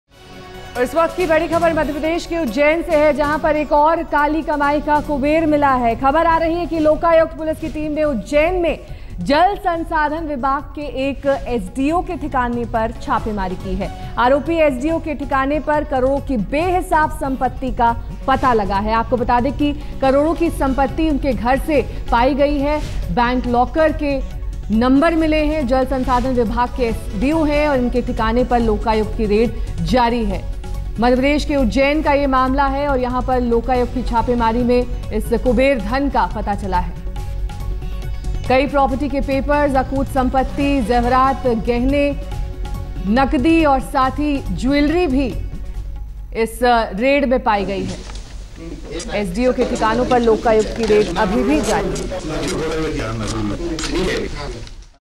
Ujjain: Lokayukta raids Water Dept SDO residence, finds crores worth of disproportionate assets
The Ujjain Lokayukta raided the residence of the Water works departments SDO and discovered crores worth of disproportionate assets. The raid is still continuing.